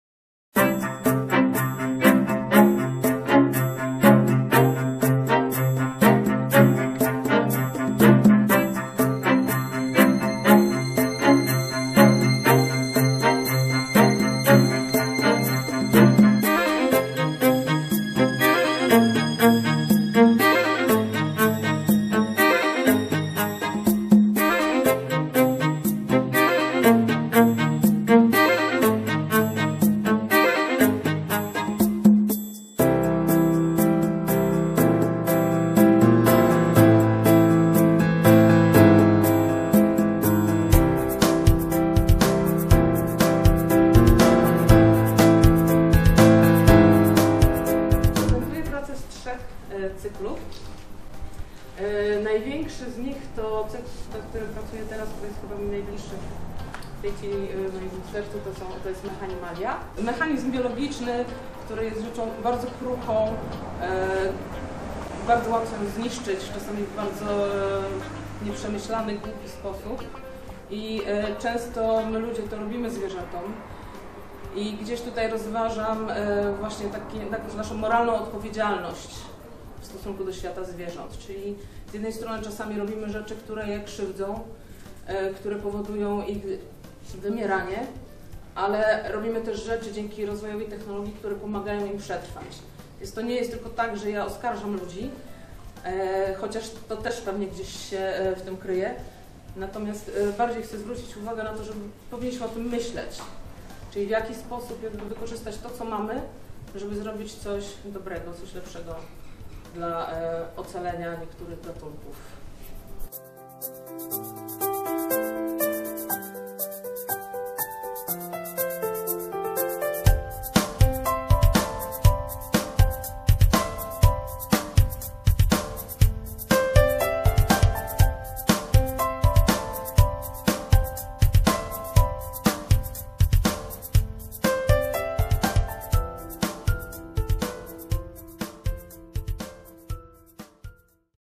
2020 07 27 Joanna Jadczak-Wodzinowska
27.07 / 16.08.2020
Wystawa prac Joanny Jadczak-Wodzinowskiej (z grupy Nie/Wypaleni) w galerii Gdyńskiego Centrum Filmowego pod patronatem medialnym Kwartalnika Artystycznego BLIZA.